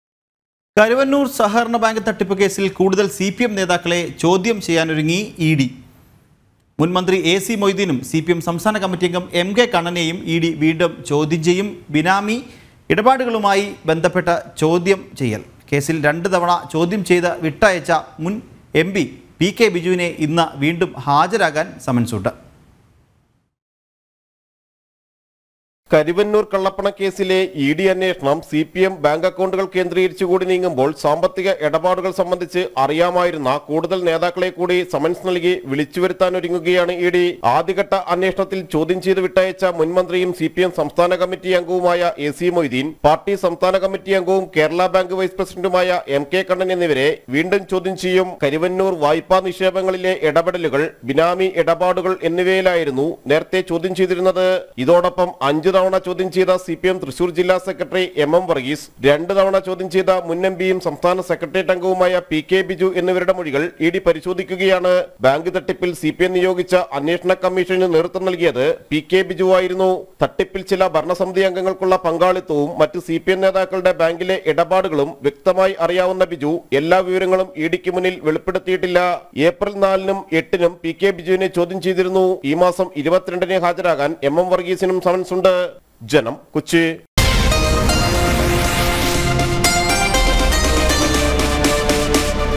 കരുവന്നൂർ കേസിൽ കൂടുതൽ CPM നേതാക്കളെ ചോദ്യം ചെയ്യാനൊരുങ്ങി ഇ ഡി |KARUVANNUR
കരുവന്നൂർ സഹകരണ ബാങ്ക് തട്ടിപ്പ് കേസിൽ കൂടുതൽ CPM നേതാക്കളെ ചോദ്യം ചെയ്യാനൊരുങ്ങി ഇ ഡി. മുൻ മന്ത്രി എ സി മൊയ്തീനെയും, CPM സംസ്ഥാന കമ്മിറ്റിയംഗം എം കെ കണ്ണനെയും ഇ ഡി വീണ്ടും ചോദ്യം ചെയ്യും. ബിനാമി ഇടപാടുകളുമായി ബന്ധപ്പെട്ടാണ് ചോദ്യം ചെയ്യൽ.
കേസിൽ രണ്ടുതവണ ചോദ്യം ചെയ്ത് വിട്ടയച്ച മുൻ MP പി കെ ബിജുവിന് ഇന്ന് വീണ്ടും ഹാജരാകാൻ സമൻസുണ്ട്.

NEWS ANCHOR : NIKHIL